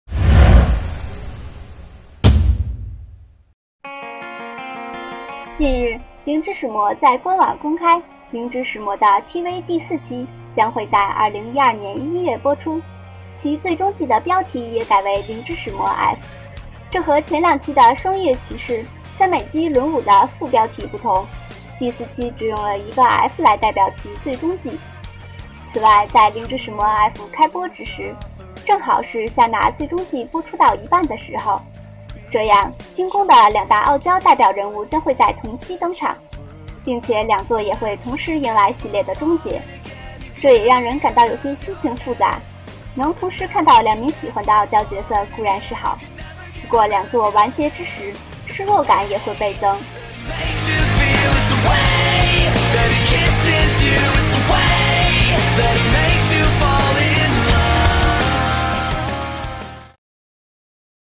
时间公布！「零之使魔」四期2012年1月开播
第四季的监督换回了第一季的监督，而小说作者山口升则负责结构构成。相信这次零使第四季会有不错的表现！